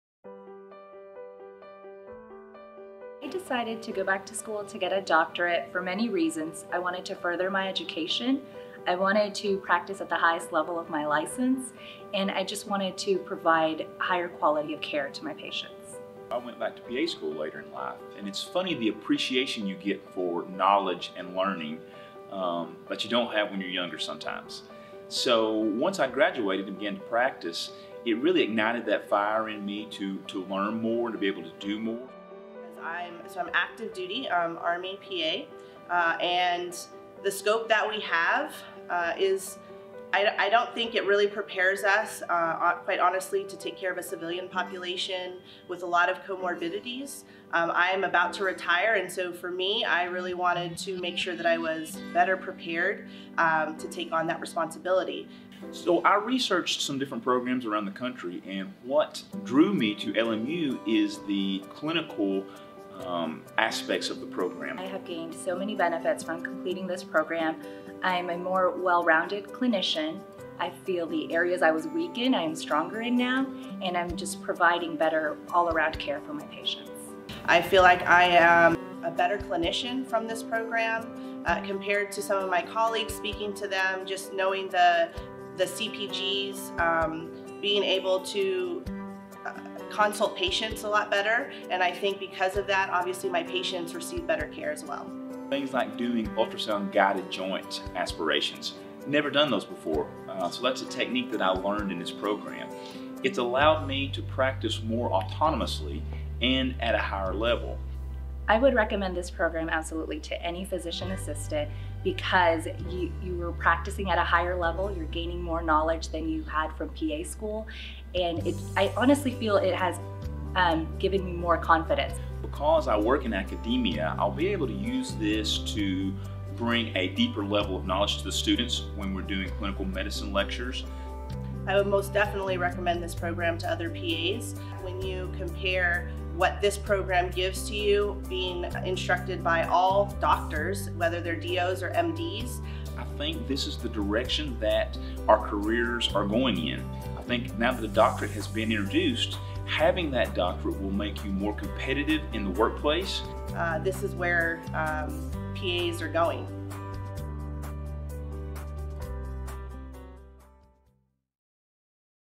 The Doctor of Medical Science Program at LMU
LMU's Doctor of Medical Science (DMS) is an advanced medical training program for physician assistants. The medical curriculum is taught entirely by physicians who are experts in their field of medicine. Utilizing a mixture of synchronous and asynchronous online didactics, the case-based curriculum design allows the students to interact with and learn from these physician experts. While enrolled in the program, students continue in their clinical and/or educational environment with their supervisor who participates in periodic competency evaluations.

SMS-DMS curriculum equips the highly skilled PA with:

- Increased depth and breadth of medical knowledge
- Increased confidence in more complex patient care
- Credentials for new leadership roles
- Advanced competence as a medical educator
- A competitive edge in the PA profession